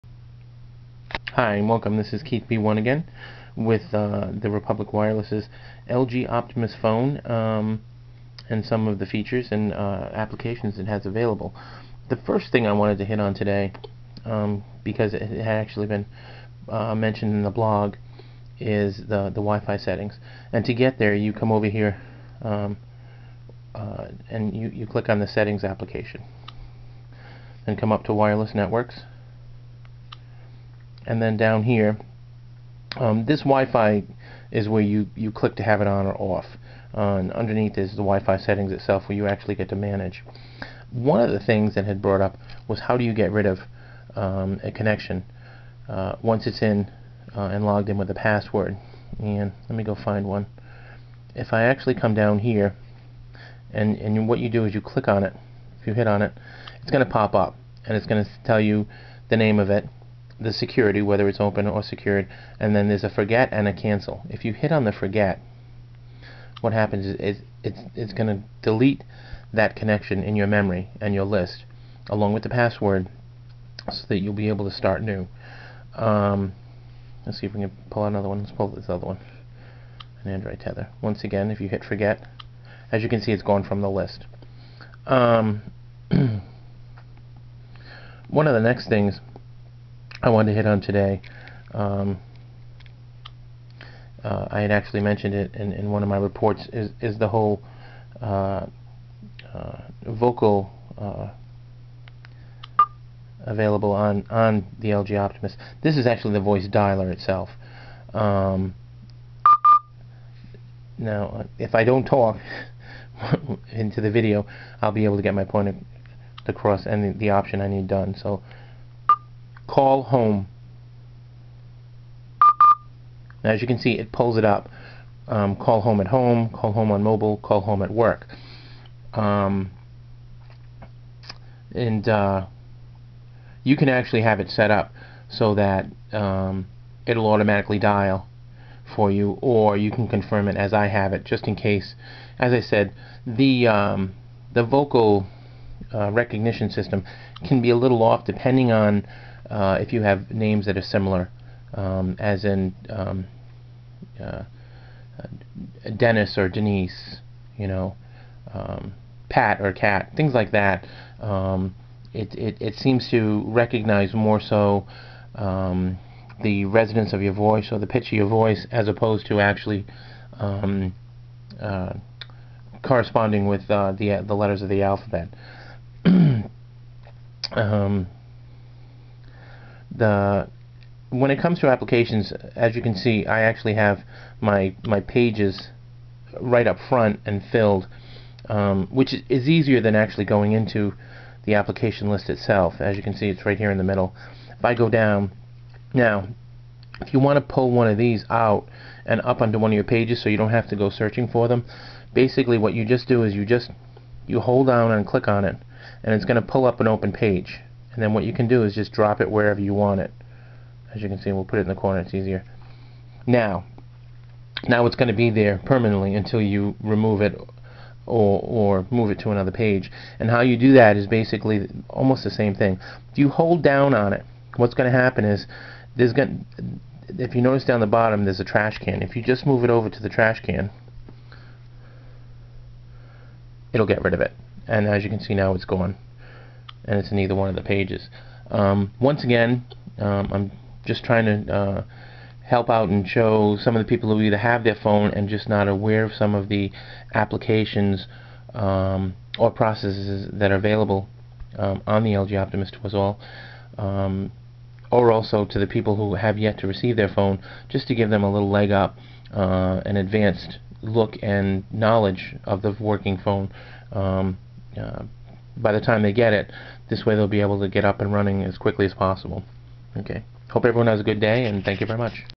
Republic Wireless LG Optimus Demo - WiFi Settings, Voice Dial, Applications Movement
Demonstration of WiFi Settings, Voice Dial app, Application Movement